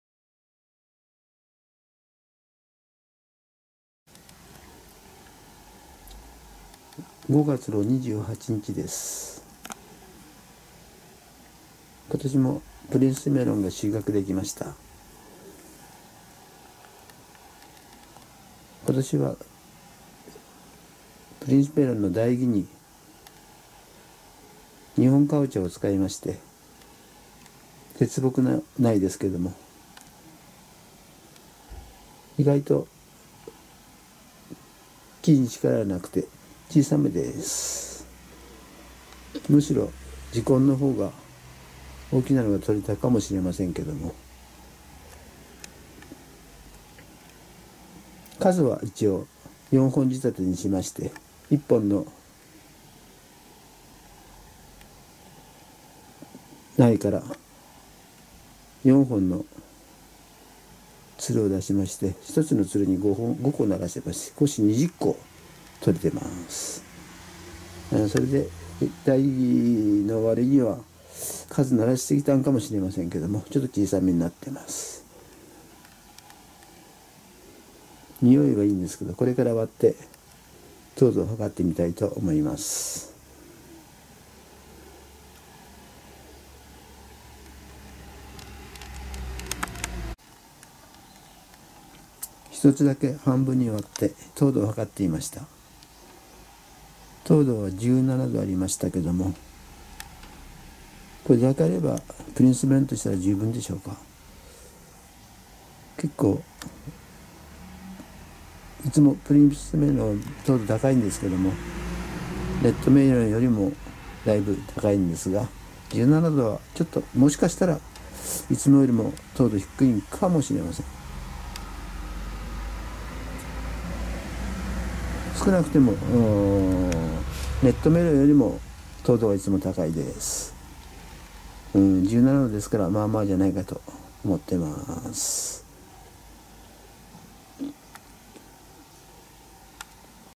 シニアおじさんのスローライフ#317（プリンスメロン収穫開始）
５月２８日
１月に種蒔したプリンスメロンの収穫を開始する